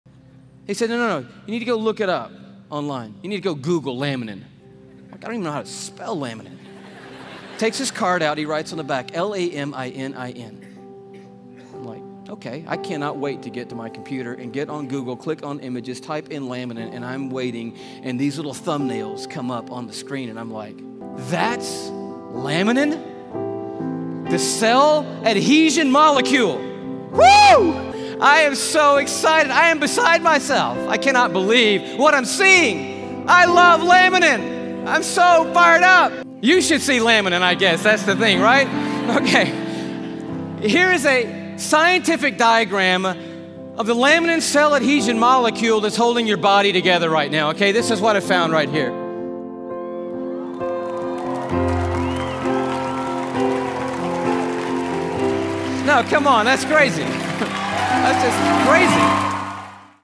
God holds us Together!
Laminin and Gods hand in the making of who we are!
Our God is good. And has a purpose for your life!